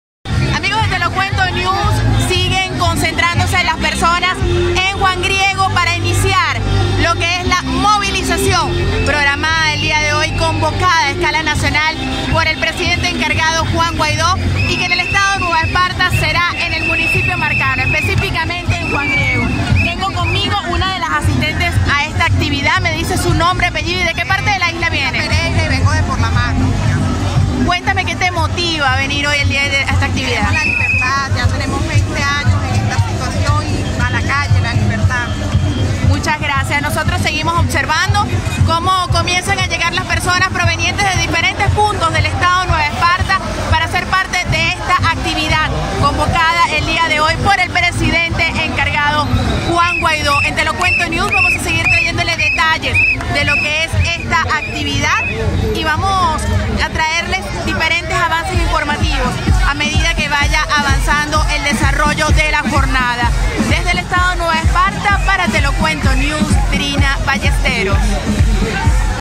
11:40 AM#2Febrero NuevaEsparta Los margariteños siguen llegando desde diferentes municipios
2Febrero NuevaEsparta Los margariteños siguen llegando desde diferentes municipios a Juan Griego, donde se llevará a cabo la gran movilización nacional convocada por el presidente (e) Juan Guaidó. Los presentes recordaron con emoción al canal pionero de Vzla: RCTV